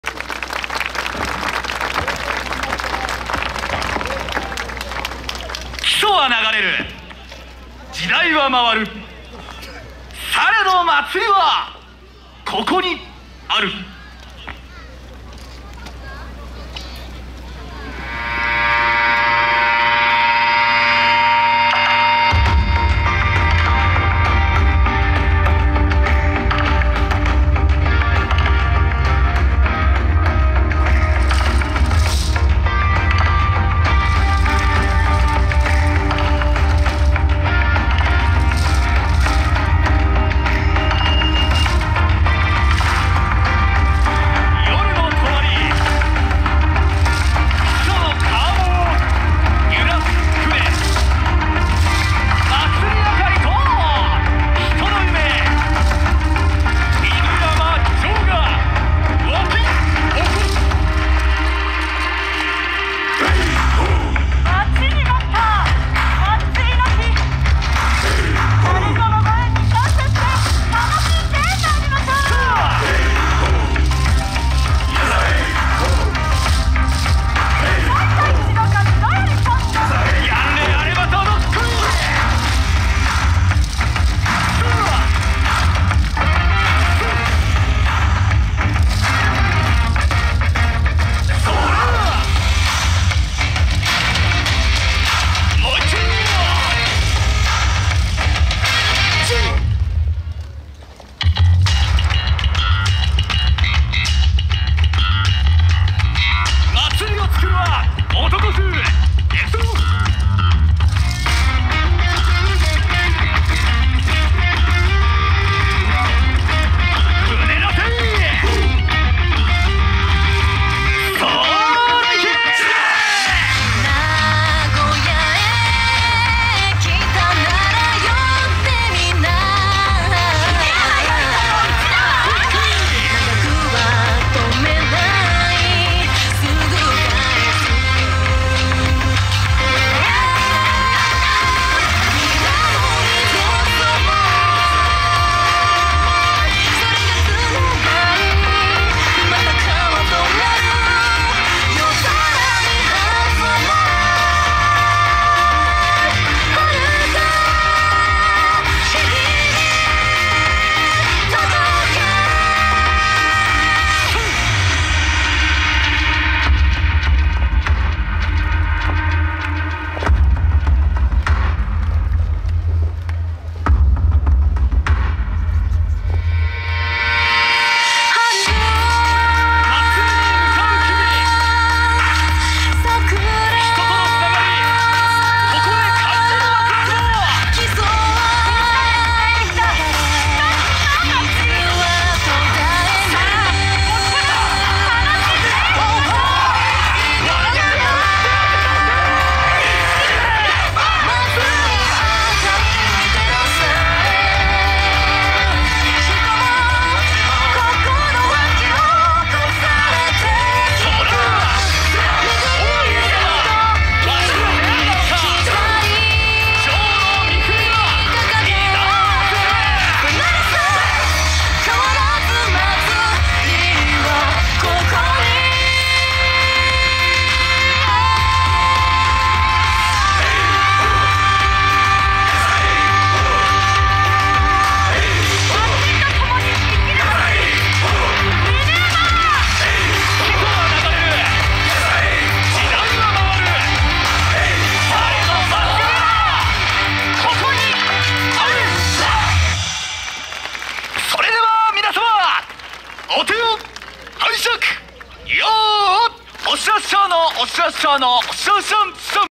笑" プレバサカニ in イオンモール土岐 2022 メイン会場 11月13日 1回目
プレバサカニ in イオンモール土岐　メイン会場にて
笑""　1回目　の演舞を撮影させていただきました。